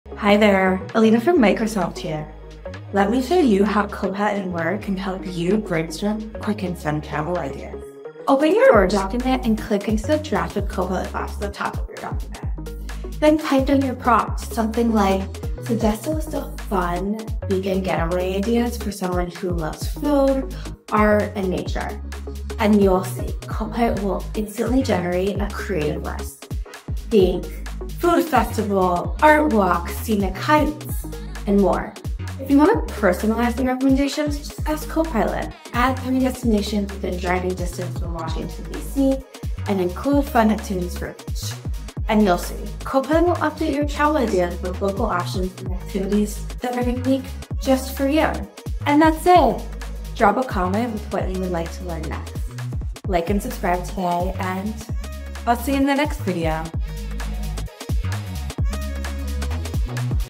Travel Ideas Made Easy with Copilot in Word | Microsoft 365
Looking for quick and fun travel ideas? Let Microsoft 365 Copilot be your tour guide.

With Copilot in Word, you can easily create the perfect list of places to go, things to see, and foods to try, all with a simple prompt. Whether you’re looking to build your travel bucket list or explore spring break travel ideas for the family, Copilot in Word can help plan your next vacation in seconds.

📝What You’ll Learn:
• How to access Copilot in Word
How to prompt Copilot for the best travel ideas
How to personalize your travel plans

Timecodes:
• 00:08 – Launch Copilot in Word
• 00:13 – Input your travel prompt
• 00:32 – Personalize for local travel options

No Microsoft 365 subscription?